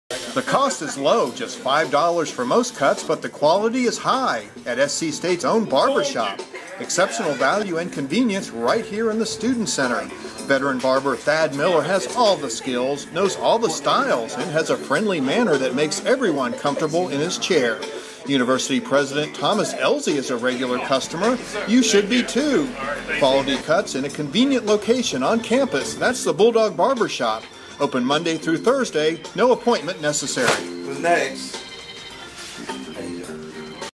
SCSU Bulldog Barber Shop Commercial
Here's a 30 second spot I shot and produced about South Carolina State University's campus Bulldog Barber Shop. Thad Miller is the barber- a great barber and great guy too!